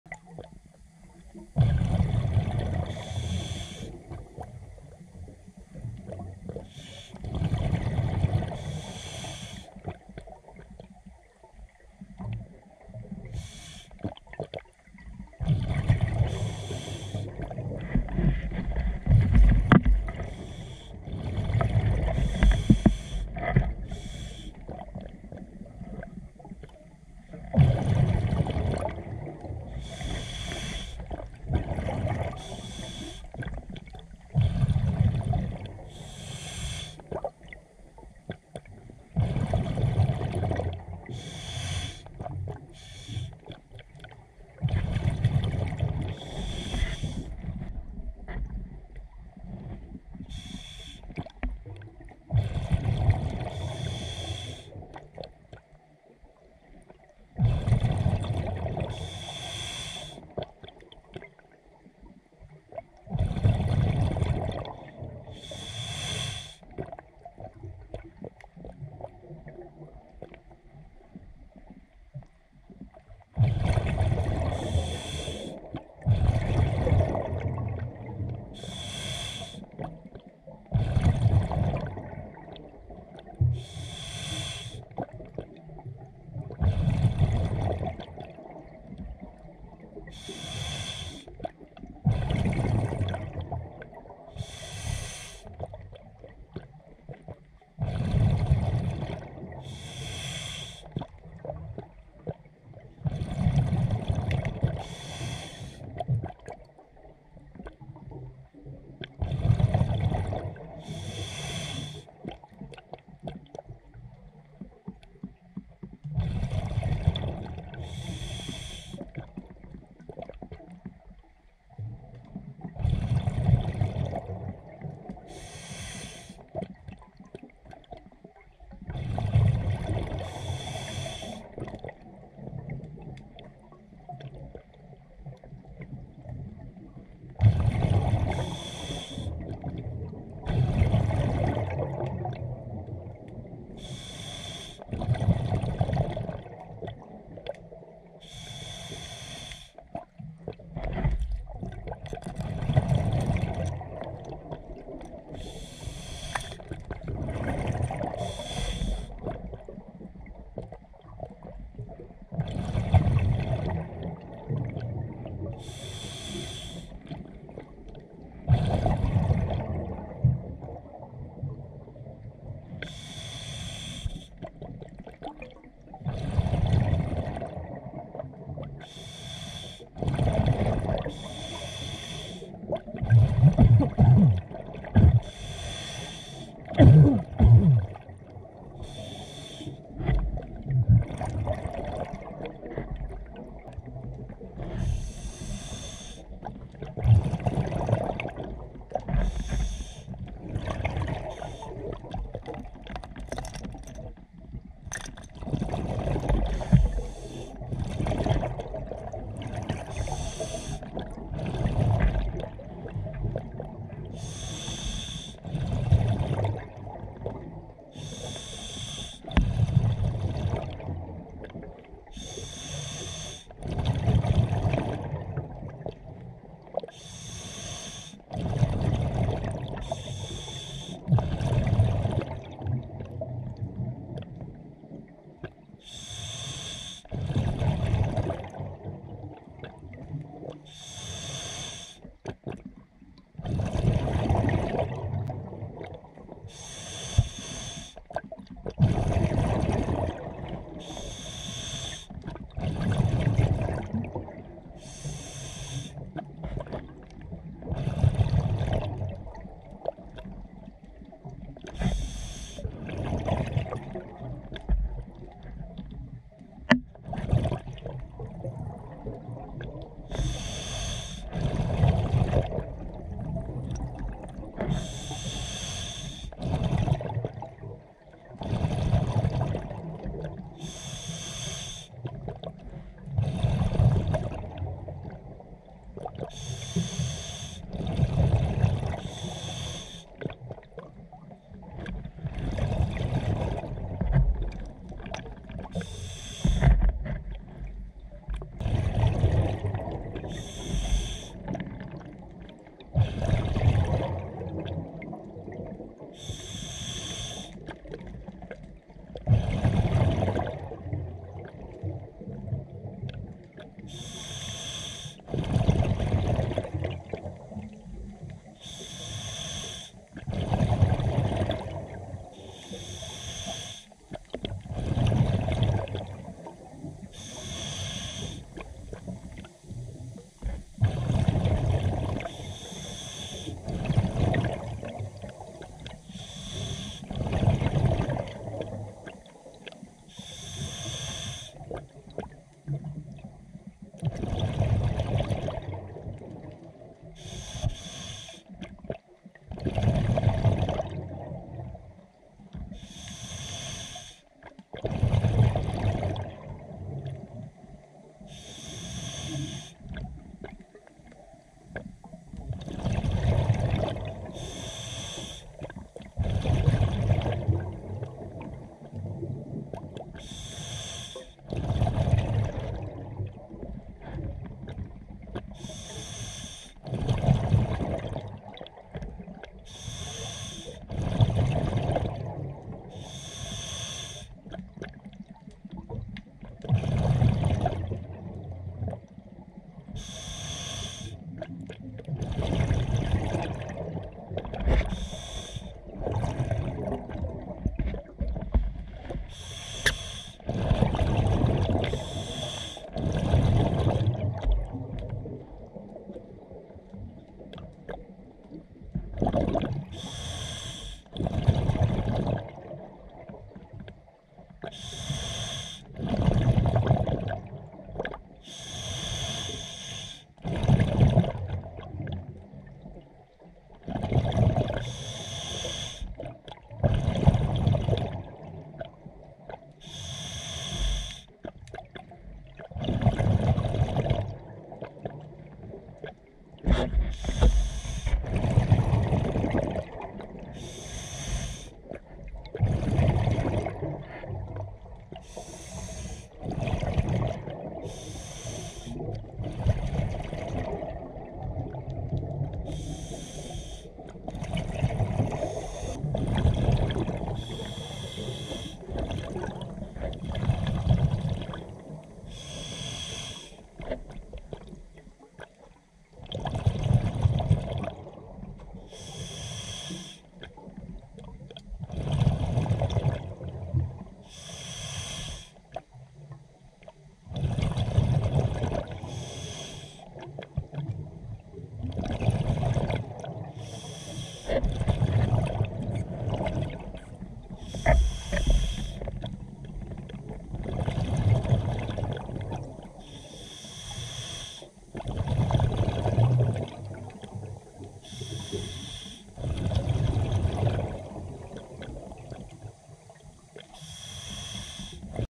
Wreck Porvariskuningas (Helsinki, Finland, #1000050351)
The 16-17 m long wreck lies in 12 m depth, close to Porvariskuningas beach in Helsinki, Finland. Some dished found in the wreck were dated to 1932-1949, but no other information on it is known.

This video is based on video material shot in one dive in Febrruary 2024. It is more documentation that art.

The Finnish Heritage Agency (FHA) id for this wreck is #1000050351, and official name for it is "Villinginsalmi".